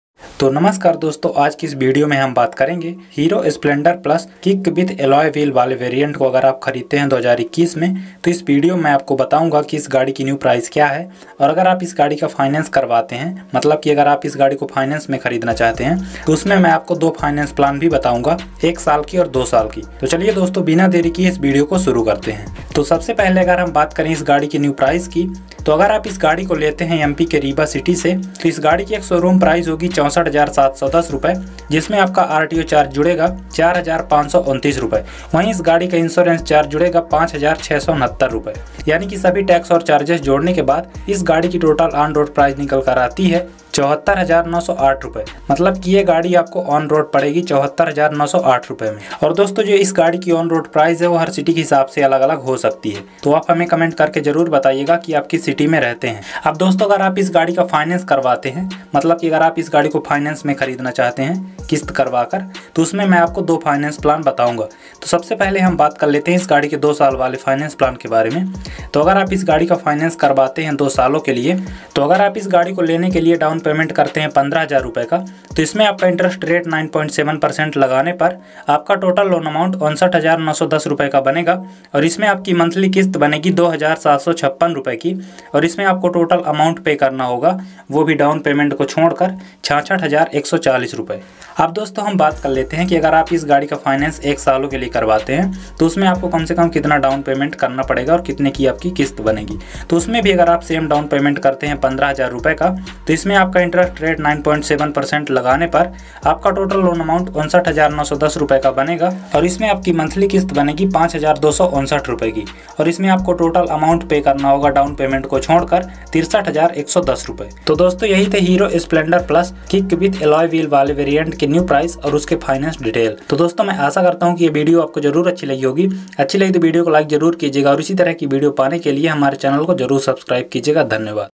New Hero Splendor Plus BS6 2021 | On Road Price | Downpayment | Finance | EMI | Full Details
To Namaskar doston aaj ki video mein humne aapko bataya hai ki agar aap New Hero Splendor Plus BS6 Limited edition ko 2021 me khareedte hai to aapko ye gaadi kitane me padegi or agar aap is gaadi ka finance karvate Hain to aapko kitana downpayment karna padega or kitne ki emi banegi sab kuch bataya hai to doston asha karta hun ki ye video aapko jarur pasand aaegi agar video pasand aaye to like jarur kijiega aur mere channel ko subscribe kijiyega dhanyawad